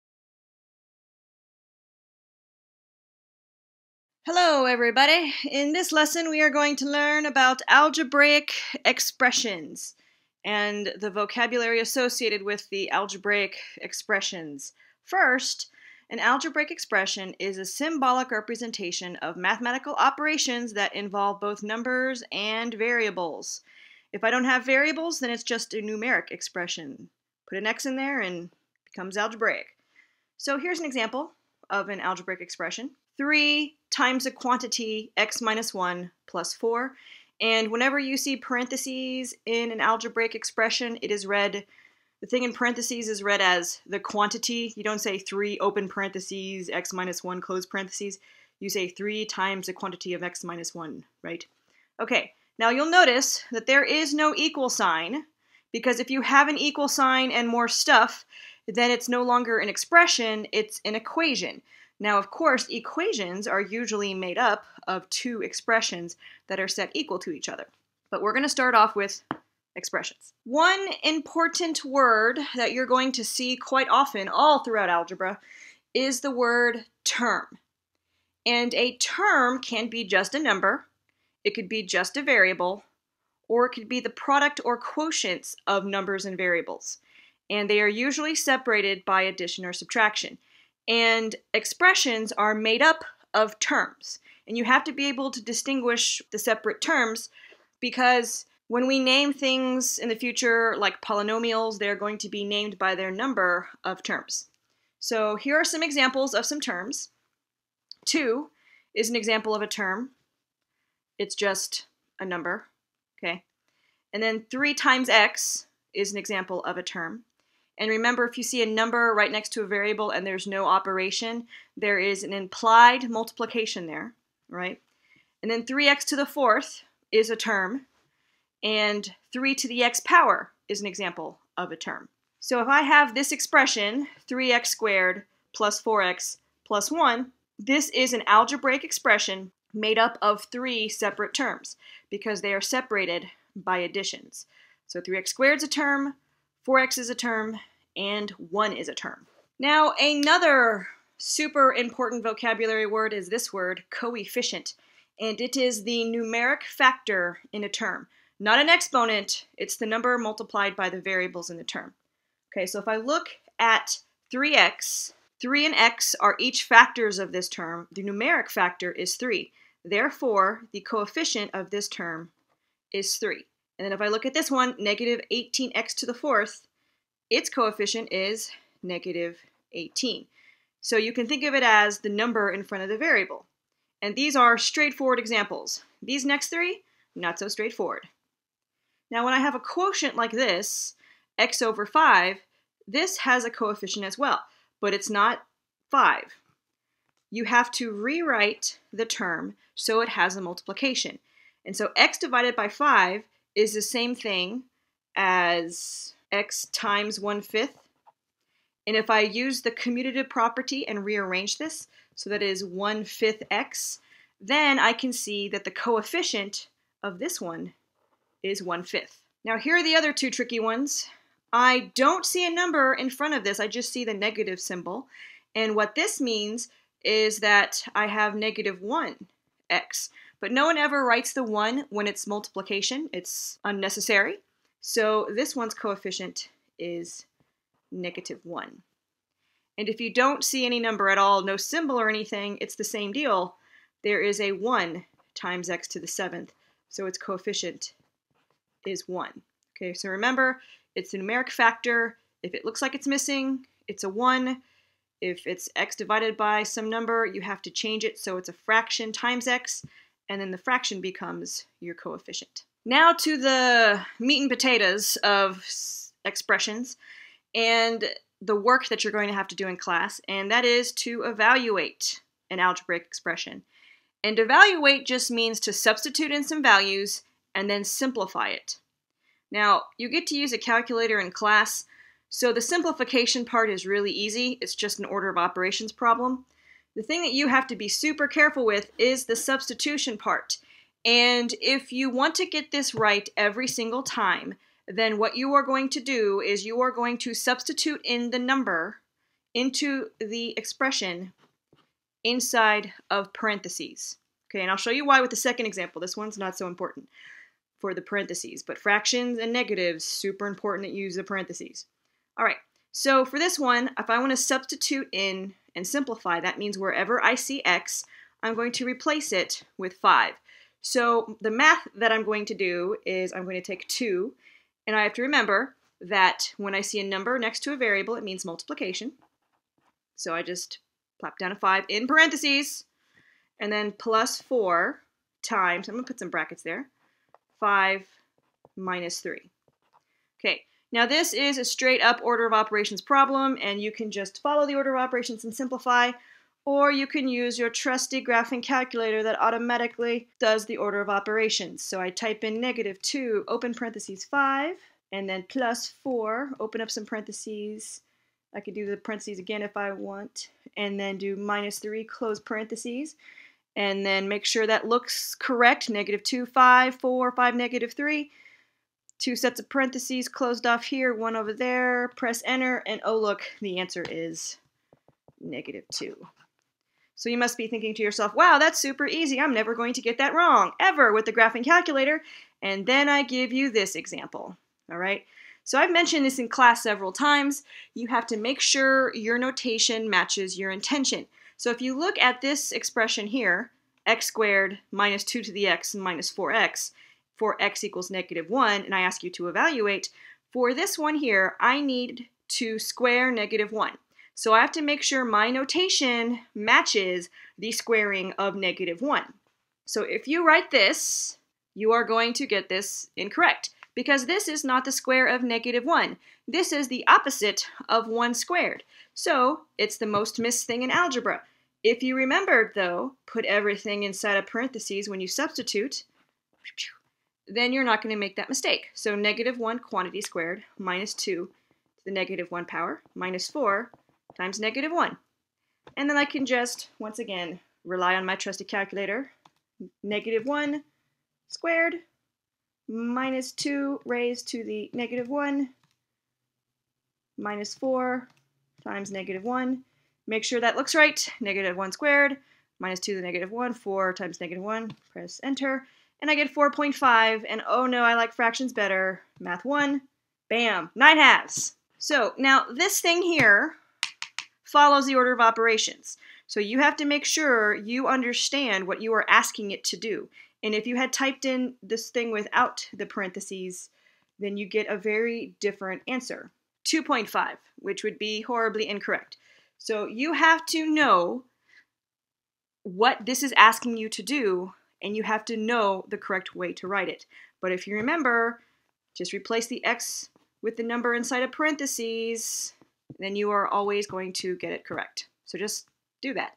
Algebraic Expressions
For Algebra 1:  In this video I go over some vocabulary associated with algebraic expressions and show you how to evaluate an algebraic expression.